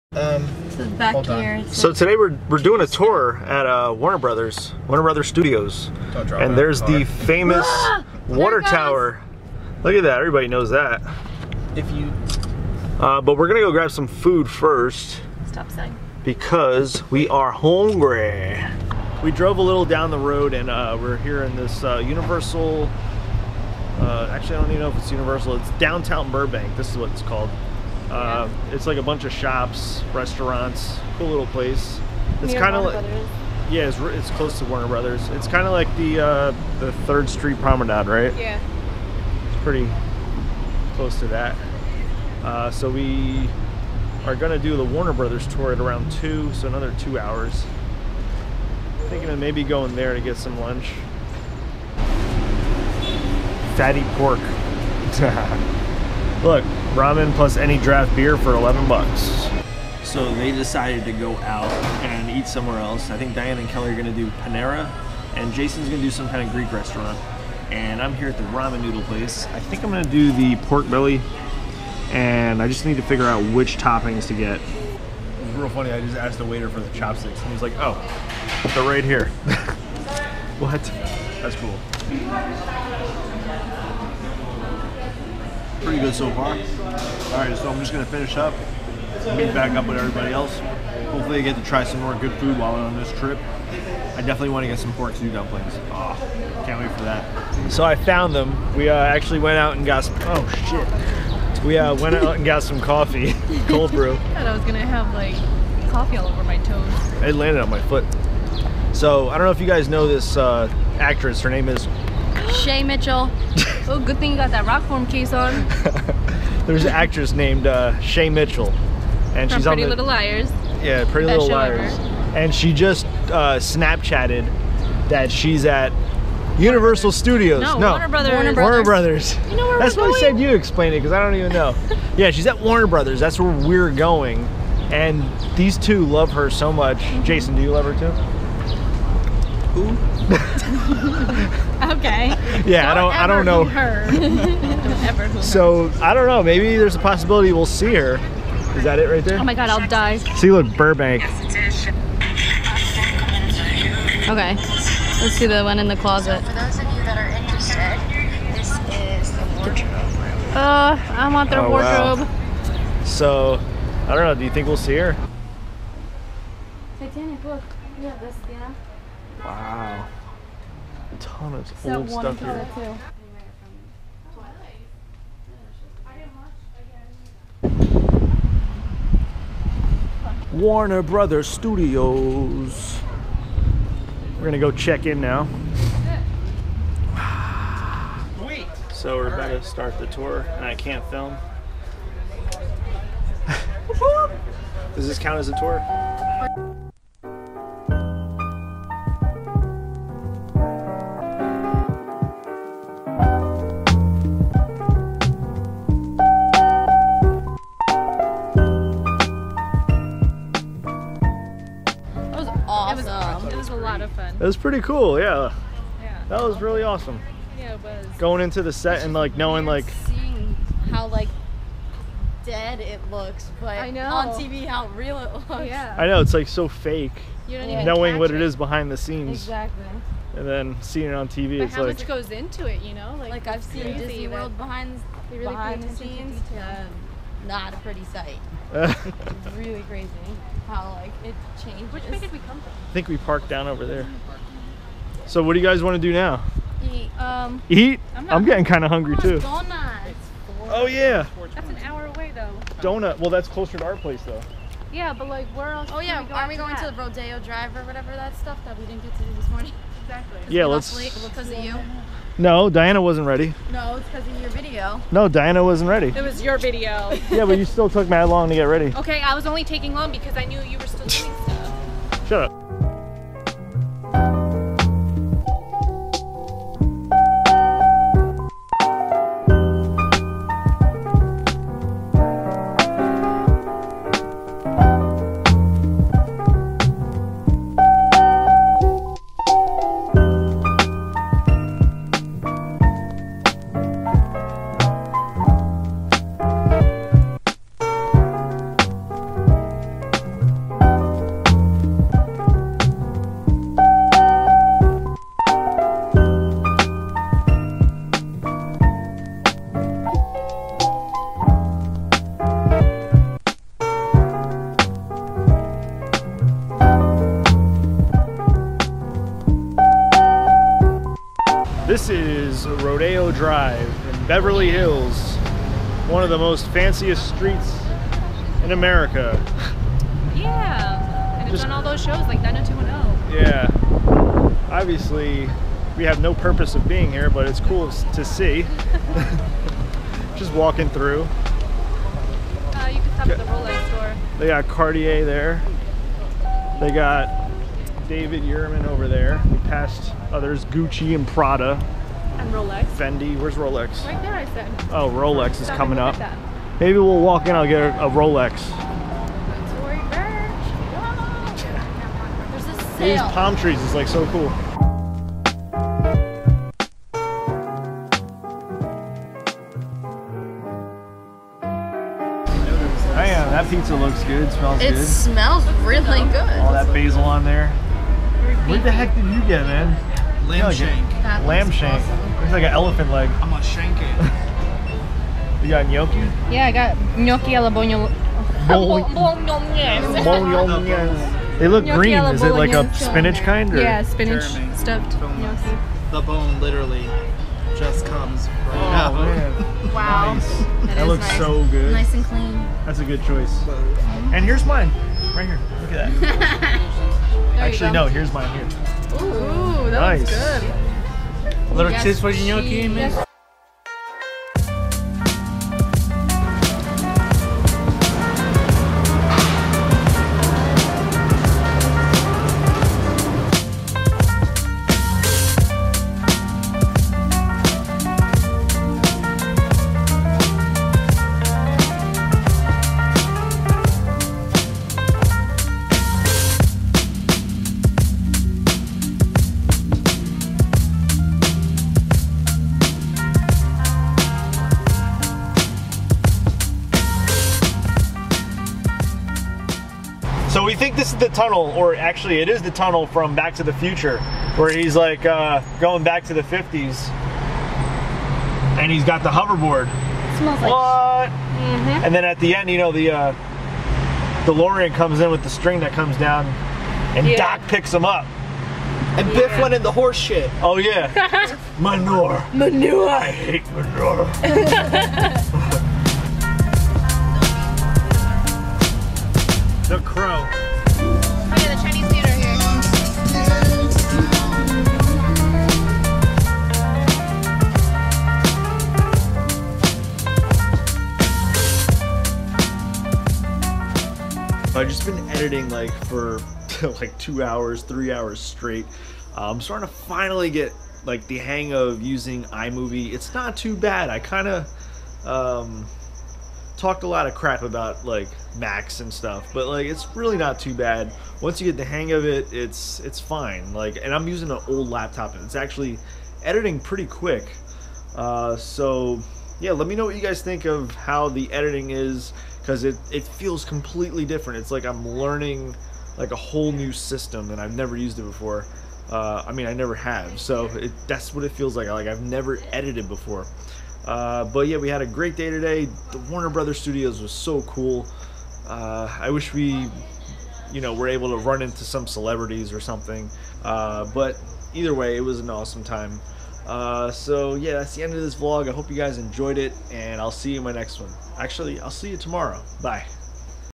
LOS ANGELES VLOG - Warner Brothers Tour and Beverly Hills
Took an awesome tour of Warner Brothers Studios, and got to see a bunch of sets from movies and shows. Drove over to Beverly Hills and walked Rodeo Drive.

MUSIC WANTED!
If you would like your music featured in my videos, then please private message me.